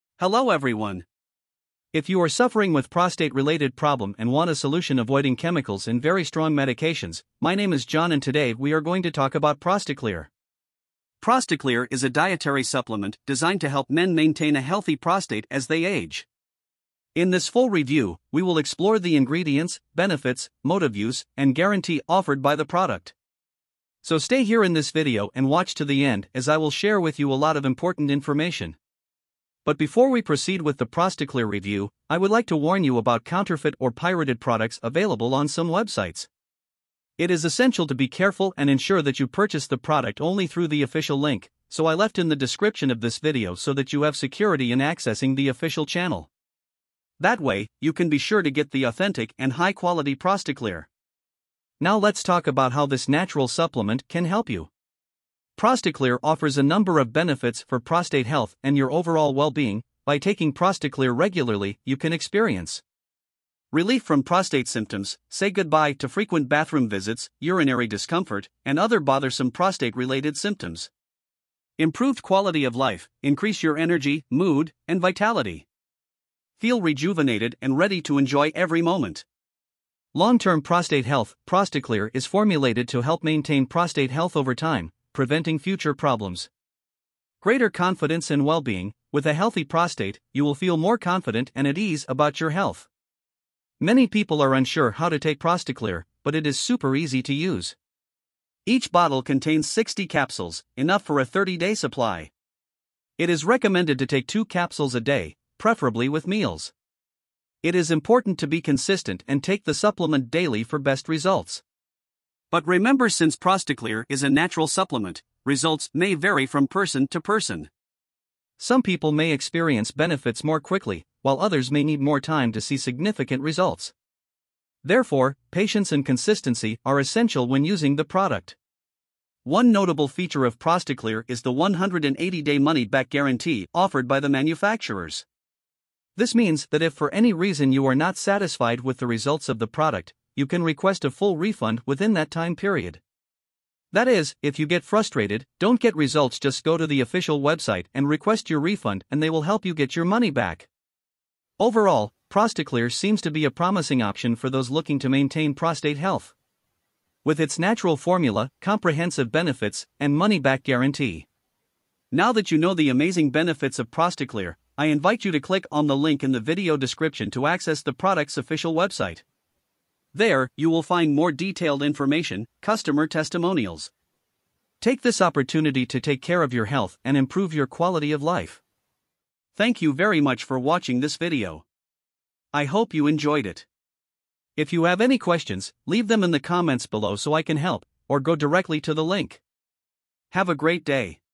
PROSTACLEAR [TRUTH REVEALED] REALLY WORKS? PROSTA CLEAR REVIEW
Looking for an effective and natural prostate solution? Join John in this captivating video review as he introduces ProstaClear, a powerful supplement designed to help you keep your prostate healthy. We will explore the ingredients, benefits, usage and guarantee offered by ProstaClear.

0:00 ProstaClear
0:08 What is ProstaClear?
0:29 ProstaClear Official Website
0:54 ProstaClear  Benefits
1:33  How to Use ProstaClear
2:08 ProstaClear Guarantee
2:31 ProstaClear  Conclusion